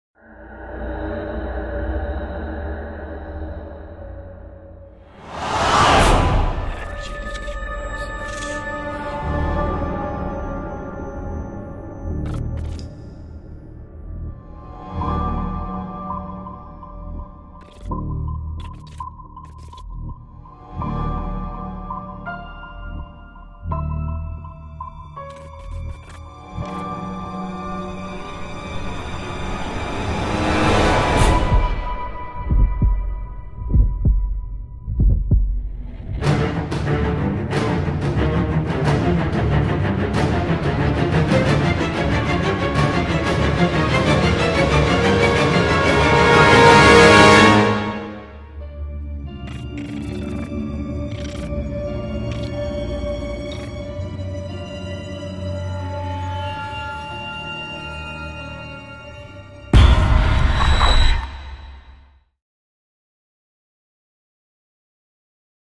Mozart Evil Spinach Trailer
The second masterpiece starring Mozart will show him having to deal with a mighty spinach bowl